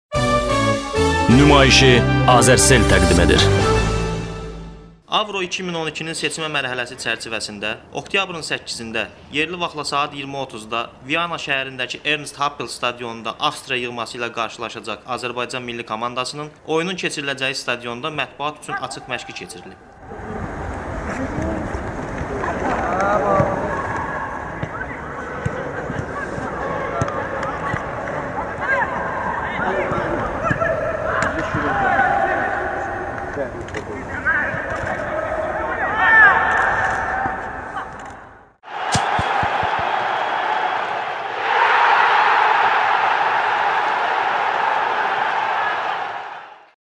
Avstriya açıq məşq
Millimiz 8 oktyabrdakı Avstriya ilə oyuna hazırlaşarkən...